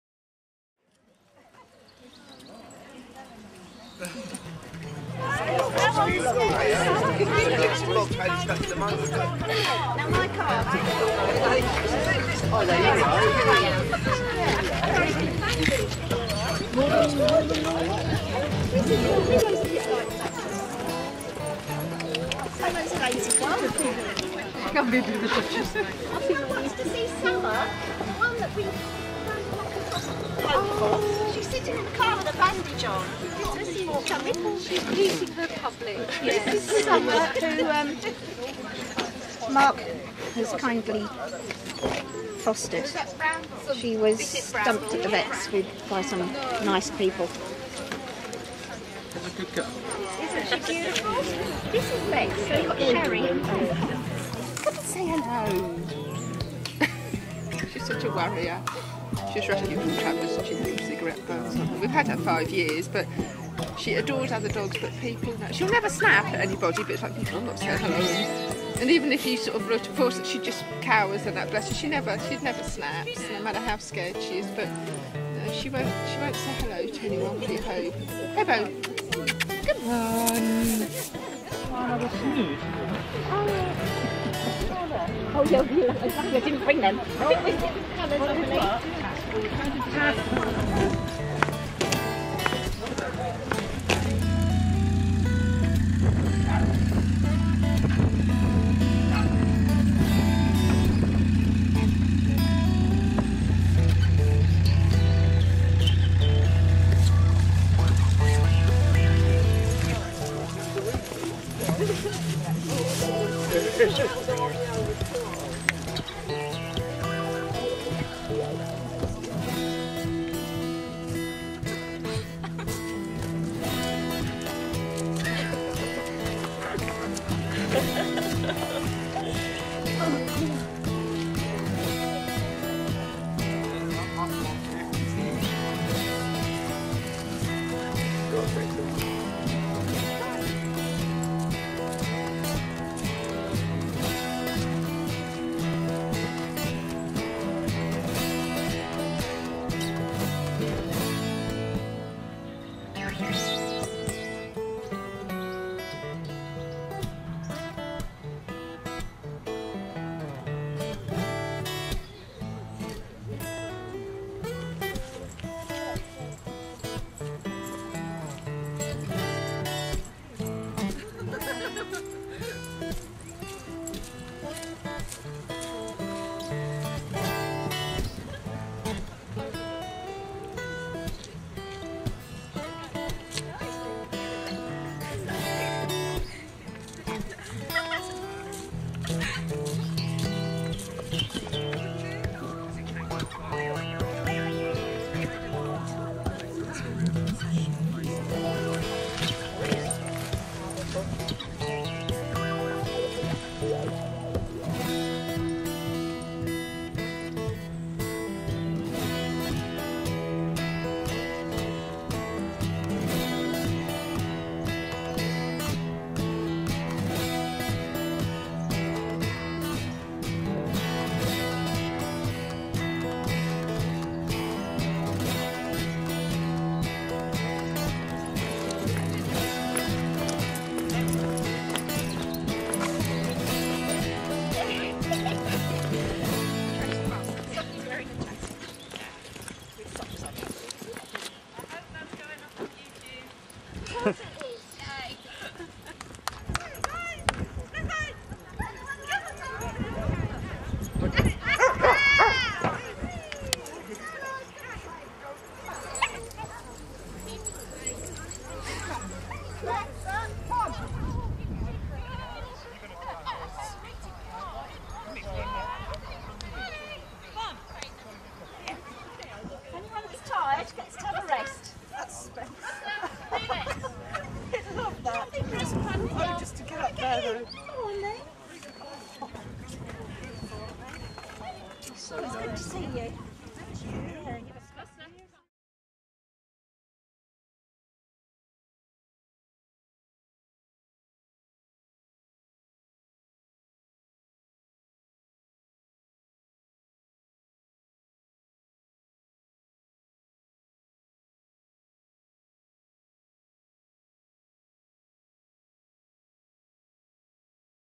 Greyhound Friends - Easter Sunday Walk April 2010
Come & join us next time!!
Picnic in the Paddock:
Stoke Bruerne - Sun 27th June 2010 at 1pm

40 wonderful dogs brought their owners on this scenic & social walk along the canal and through the surrounding Stoke Bruerne countryside. Afterwards, the energetic ones had lots of fun in the paddock chasing each other and racing Peter (a daft human) across the finish line. What a fabulous sight to see, and as a result of everyone's support and kind generosity on the day, we were able to send £175 to Tia Greyhound & Lurcher rescue!

Event organised by Alison, Marian & Jeni of Greyhound Friends.

Camera work by Peter Walsh, Gabrielle Le Cocq & Paul Staufert
Edited by Gabrielle Le Cocq